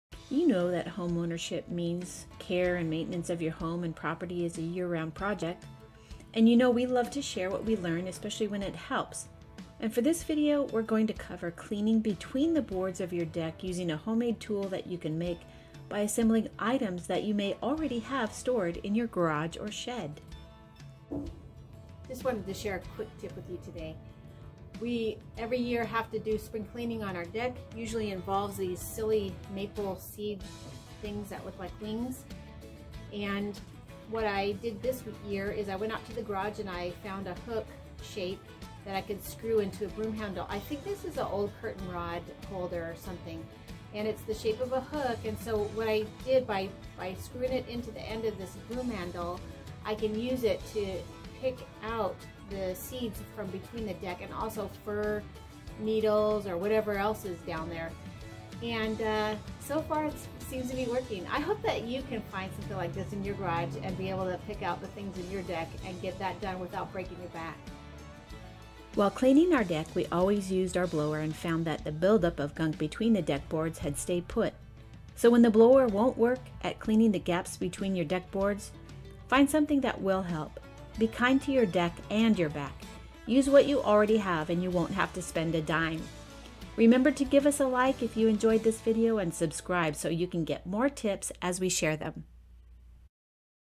Clean Deck Grooves and Gaps - NO COST Solution
We love to share what we learn, especially when it helps. While cleaning our deck, we always use our blower and have found that the build-up of gunk between the deck boards stays put. So when the blower doesn't clean the gaps between deck boards, it's time to find a solution!
This video covers cleaning between the boards and in the gaps of your deck using a homemade tool that you can make by assembling items you may already have stored in your garage or shed. Use what you already have and you won’t have to spend a dime.